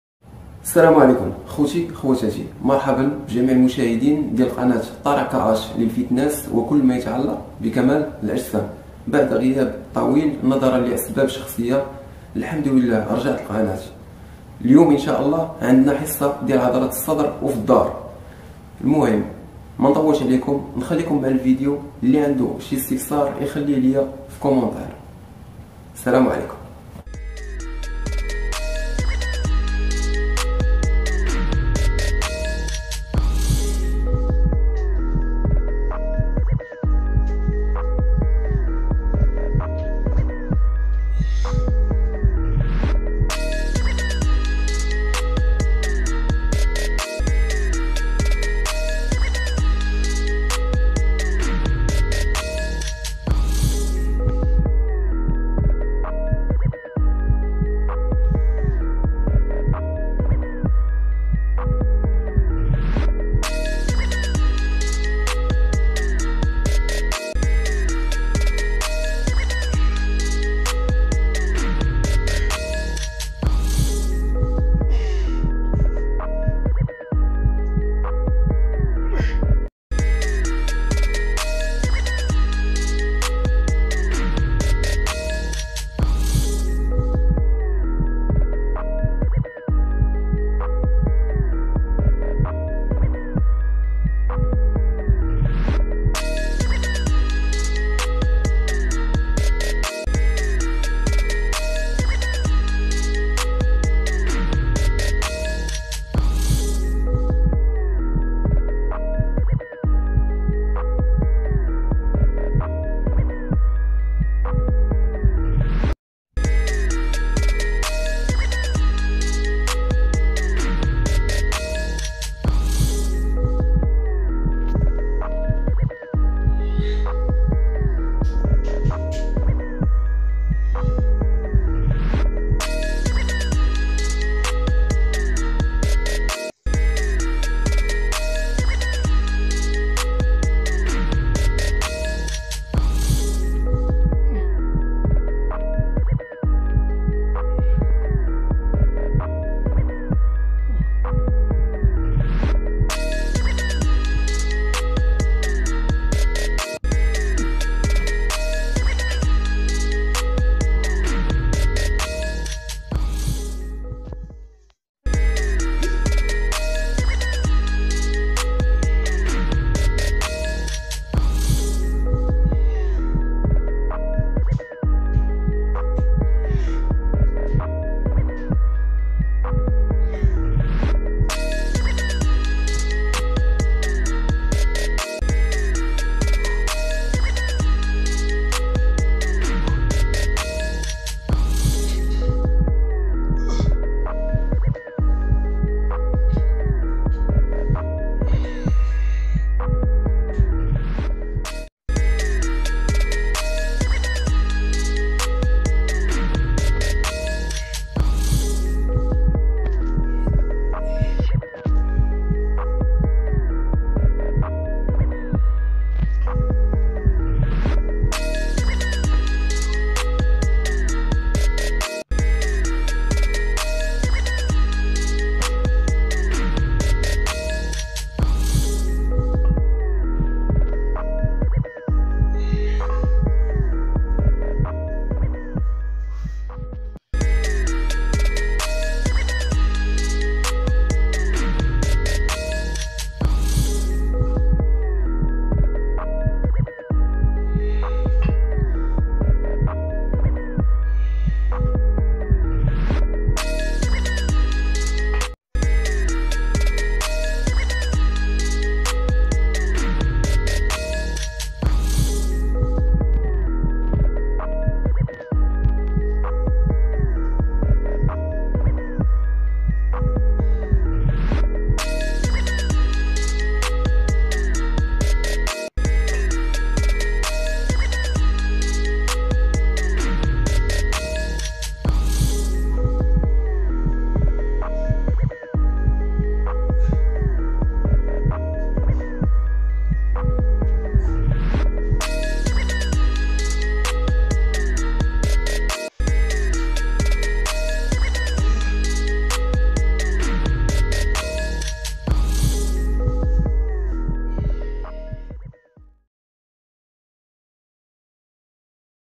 برنامج لتضخيم عضلة الصدر 🔥🔥
فهاد الفيديو غنوريكوم كيفاش ترينيو الصدر بطريقة صحيحة بلابار وليزالطير فقط وغير فالدار بحيث:

تعتبر عضلة الصدر من أهم العضلات في جسم الإنسان، فهي تلعب دورًا كبيرًا في عملية التنفس وأيضًا في حركات الكتف، وتكون على شكل طبقات عضلية قليلة السماكة وألياف تتحد مع بعضها على شكل وتر يتصل بالكتف.
يبحث جميع الرجال عن أفضل التمارين التي تعمل على تقوية عضلة الصدر، لأنها تعطي جمال و جاذبية للجسم . لذلك قمت بجمع مجموعة من التمارين الرياضية التي تعمل على تحسين شكل عضلات الصدر وبناء شكل قوى لها للمبتدئين